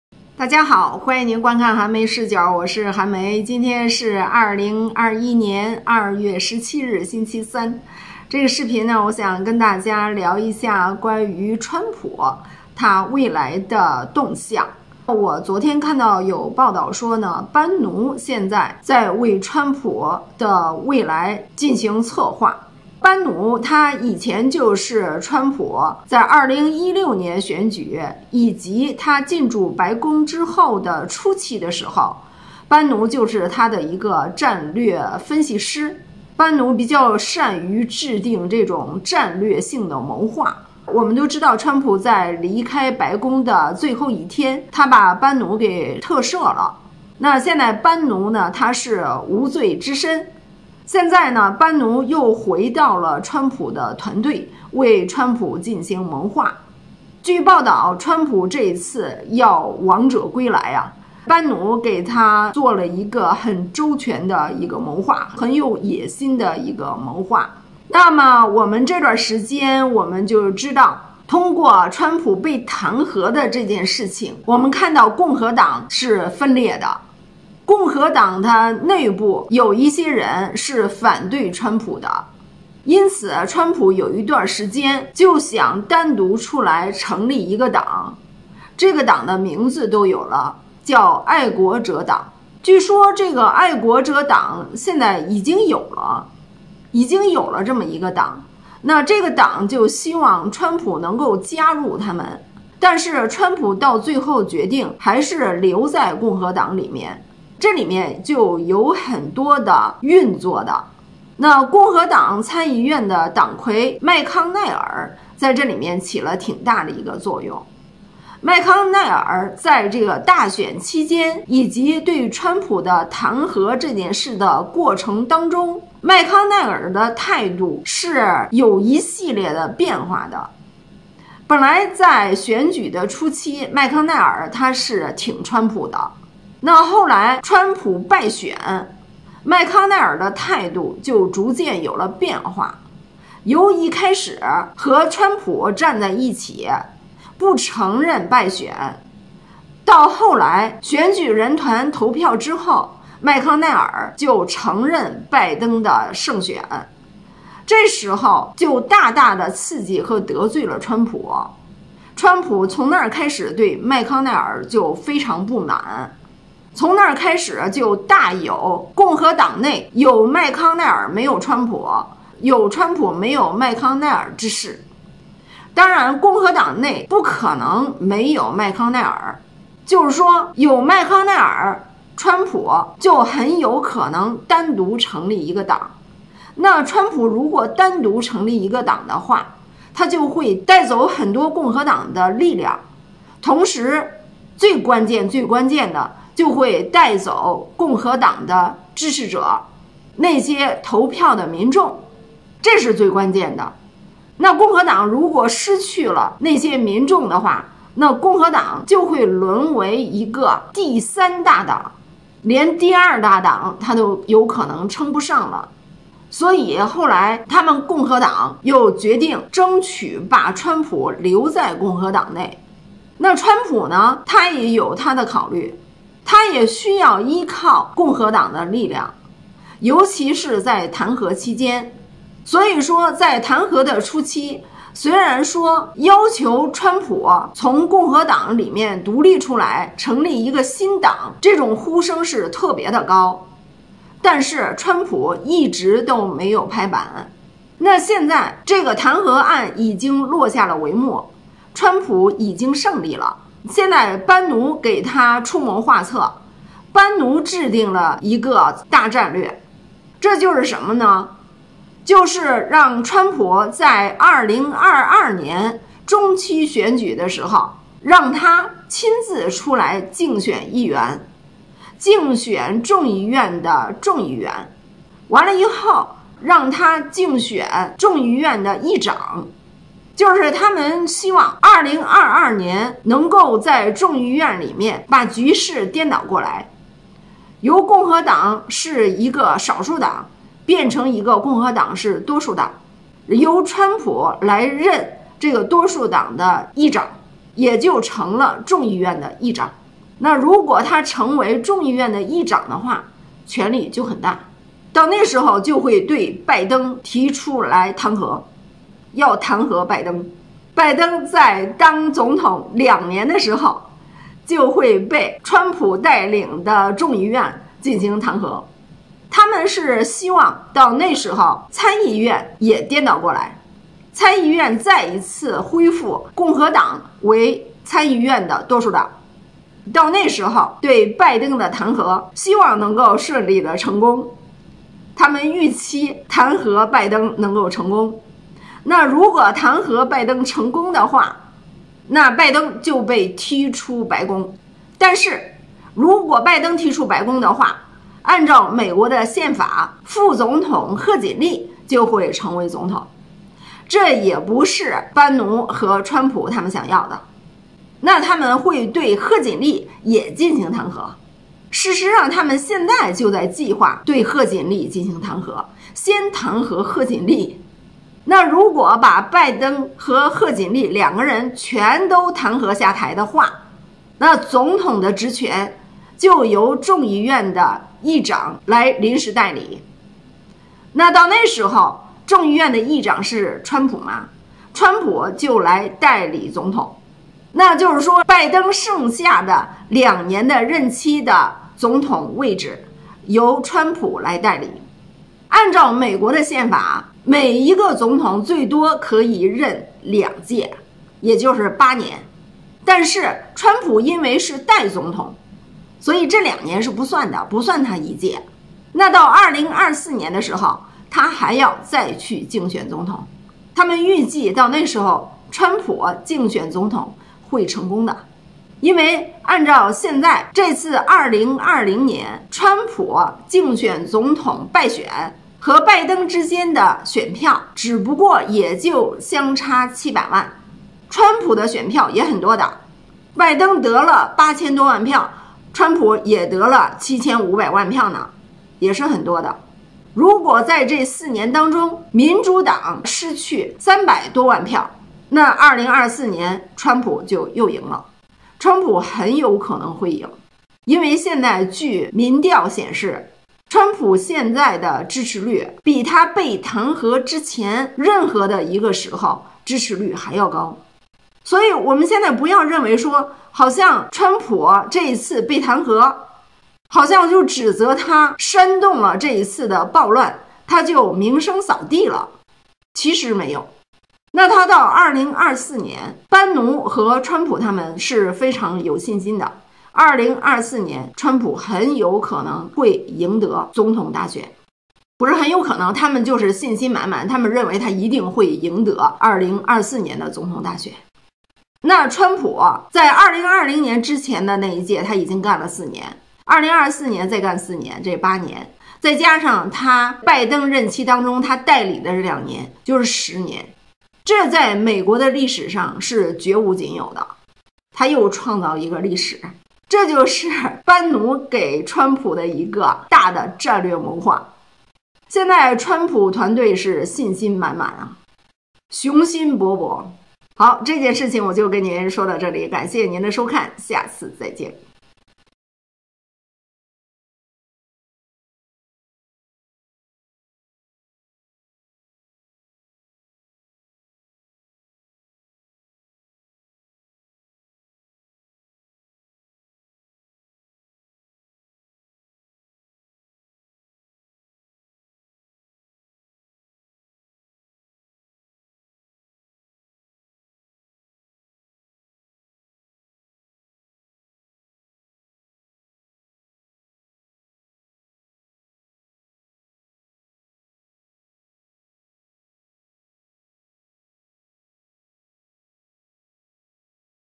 20210217-2【全面布局】【班农为川普谋划：中期篡权国会，2024总统大选】【寒梅视角】【中文字幕】
每个视频最后都有我的微信二维码，微信红包二维码， 支付宝二维码， 感谢您打赏的支持！